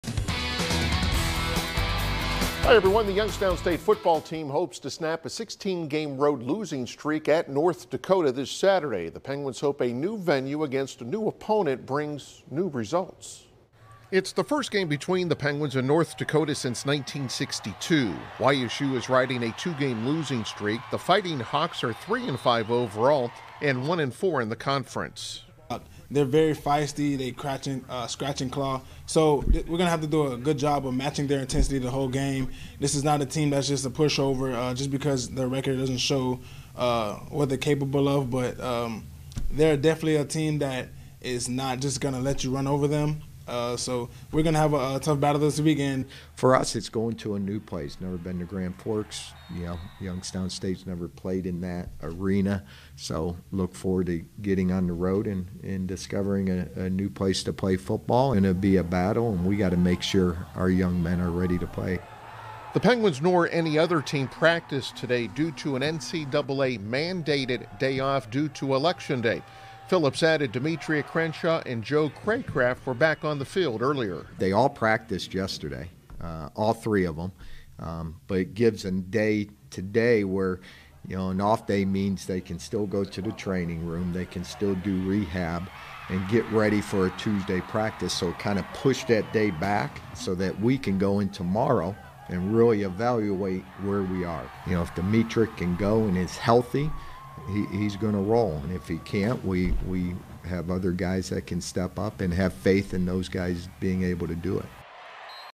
YSU Report: Penguins travel to North Dakota on Saturday
The Youngstown State football team hopes to snap a 16 game road losing streak at North Dakota this Saturday. It's the first game between the Penguins and North Dak